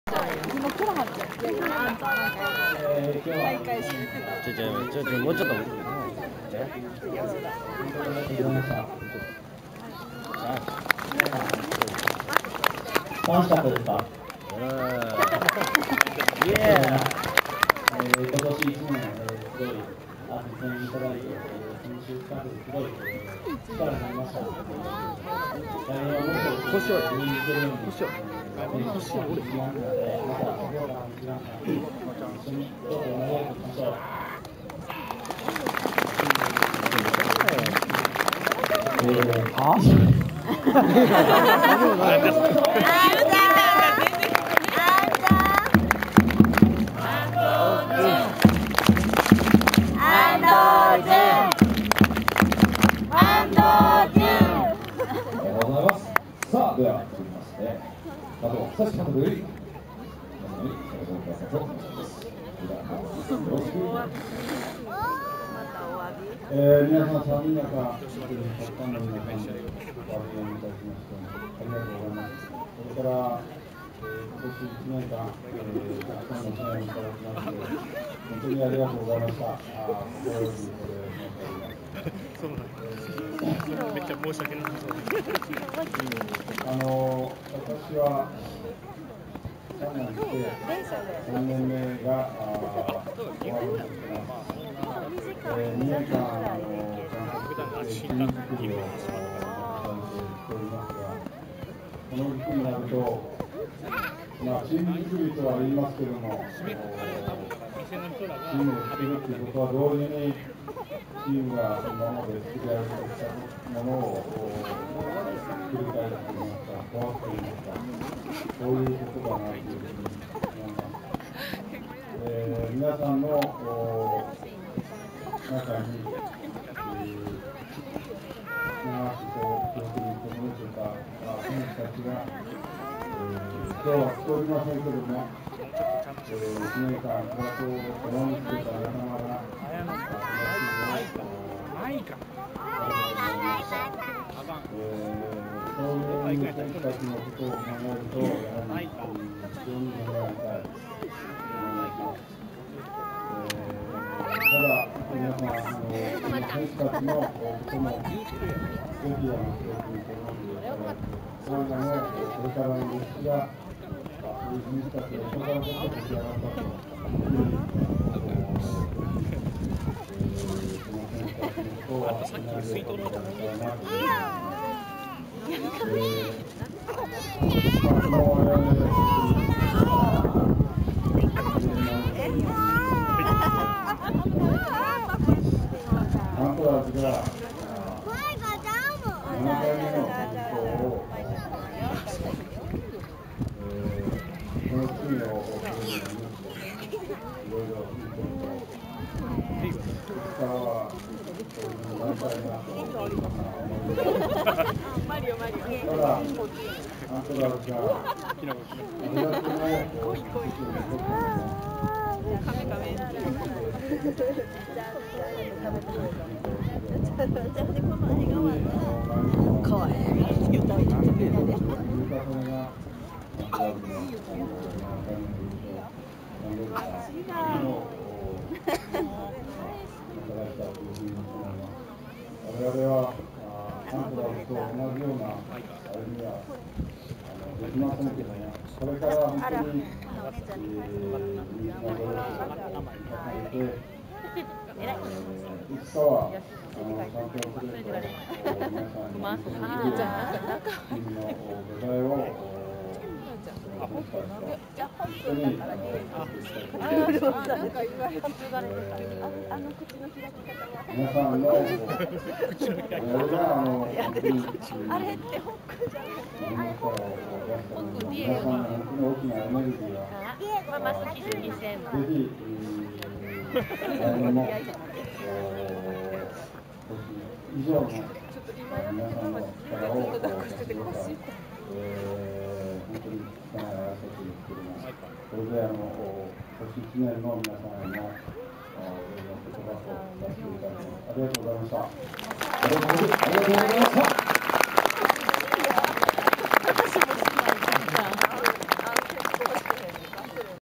2009 京都サンガファン感謝祭けり納め 閉会式１
０９１２０６　京都サンガファン感謝祭　於サンガタウン。安藤選手の挨拶－加藤監督挨拶。Ｑさん話長すぎ。しかもボソボソしゃべるので聞き取り辛くて